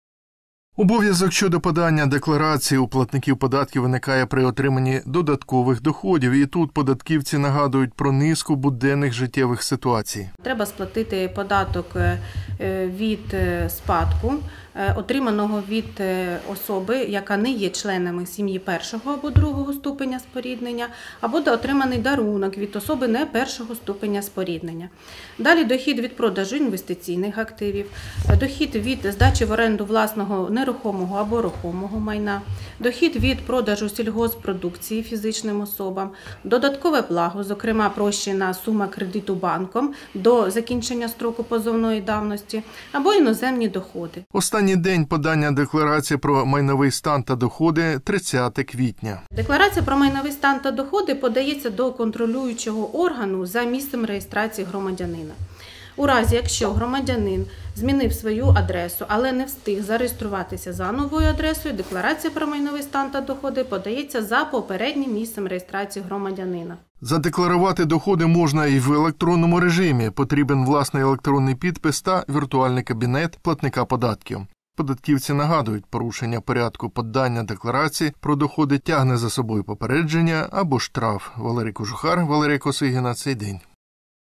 Доходи - офіційно: кампанія декларування громадянами доходів завершується в останній день квітня
В Україні триває декларування громадянами доходів, отриманих упродовж минулого року. Фахівці Криворізького північного відділу державної податкової служби нагадують городянам - до завершення кампанії залишилося трохи більше трьох тижнів.